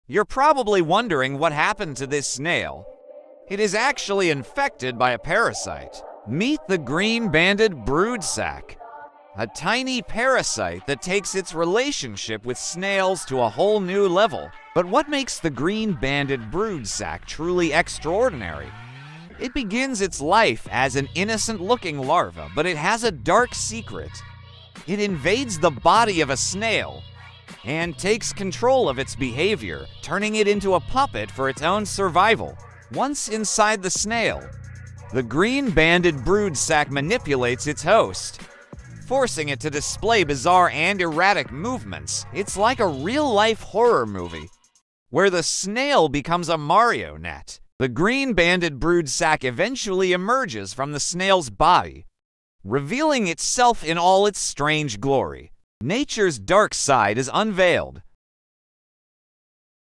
Meet the Disco Zombie Snail
The green-banded broodsac refers to a parasitic infection caused by a trematode parasite called Leucochloridium paradoxum. It primarily affects snails, particularly species of the genus Succinea.

The life cycle of Leucochloridium paradoxum involves infecting snails and using them as intermediate hosts. The adult trematodes live in the digestive system of birds, their definitive hosts. The parasite releases its larvae, known as cercariae, into the snail's slime trail. These larvae are then consumed by birds when they peck at the slime trail.

The interesting aspect of this parasite is that it manipulates the behavior of infected snails to increase the chances of being consumed by birds. The broodsacs of Leucochloridium paradoxum develop in the snail's eyestalks, forming swollen, green, and pulsating structures that resemble caterpillars or larvae. These broodsacs are visually striking and attract the attention of birds.

The infected snail, under the influence of the parasite, exhibits abnormal behavior. It becomes more active during the day, instead of its usual nocturnal behavior, and tends to crawl to exposed areas, making it more visible to birds. This behavior is thought to be an adaptive strategy of the parasite to increase the likelihood of being consumed by a bird, which is necessary for the completion of its life cycle.

Once a bird consumes an infected snail, the broodsacs release their larvae in the bird's digestive system. The larvae then develop into adult trematodes, and the cycle continues when the bird excretes the parasite's eggs, which are then consumed by snails.

The green-banded broodsac phenomenon is fascinating and provides an example of the complex strategies parasites employ to ensure their survival and reproduction.